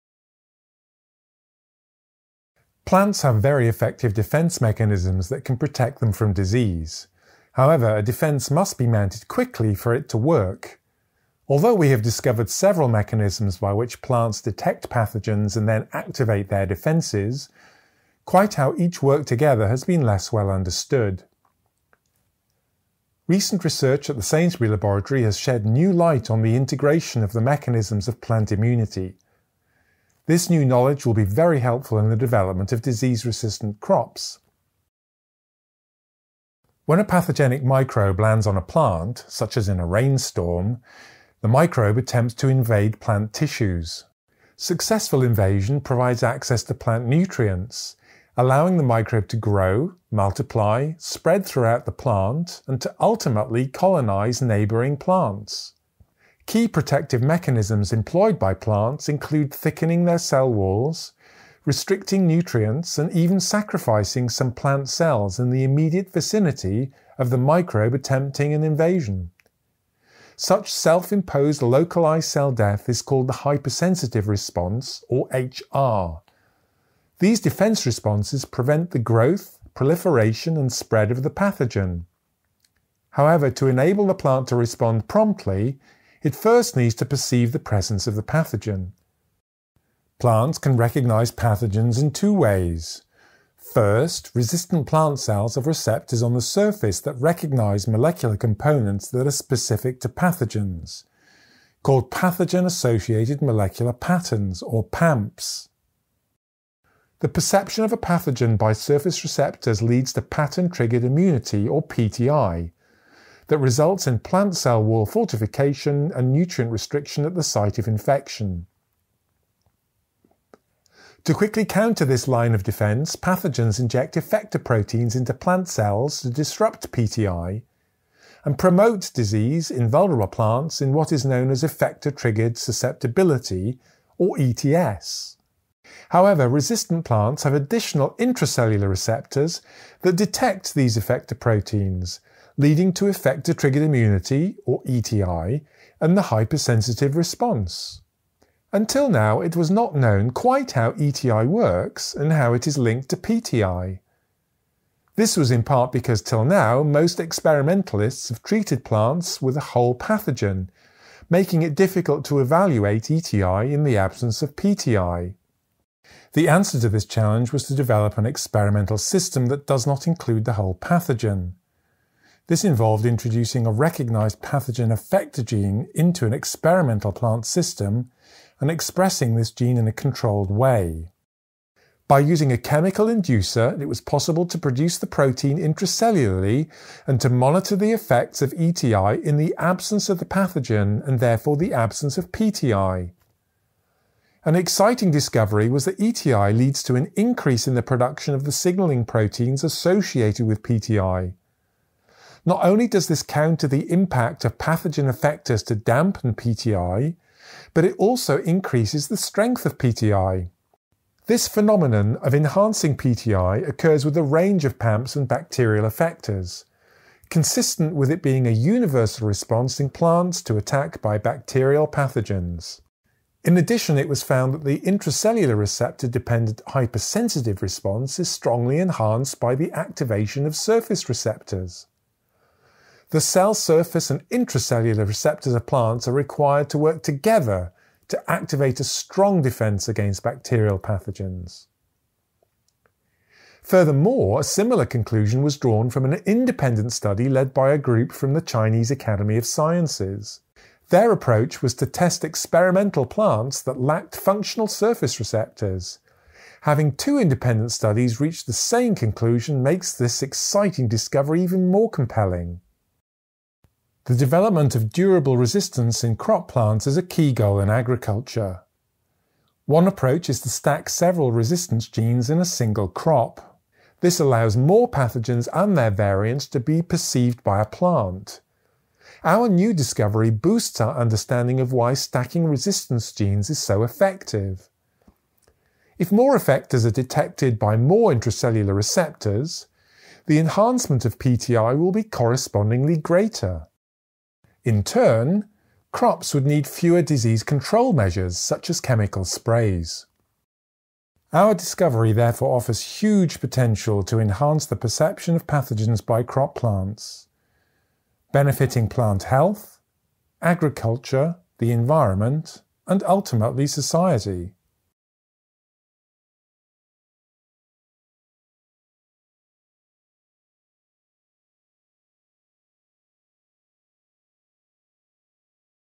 Plant immunity requires two-step detection of invaders
Cell-surface and intracellular receptors of plants work together to activate a strong defence against bacterial pathogens. Scientists at The Sainsbury Laboratory made this exciting discovery. This new knowledge will help the development of new crops that are resistant to disease, reducing the need for chemical treatments.

Bruno Pok Man Ngou, Hee-Kyung Ahn, Pingtao Ding & Jonathan D. G. Jones. Mutual potentiation of plant immunity by cell-surface and intracellular receptors. Nature (2021).

Minhang Yuan, Zeyu Jiang, Guozhi Bi, Kinya Nomura, Menghui Liu, Yiping Wang, Boying Cai, Jian-Min Zhou, Sheng Yang He & Xiu-Fang Xin. Pattern-recognition receptors are required for NLR-mediated plant immunity. Nature (2021).

Elements of the animations were created with BioRender.com.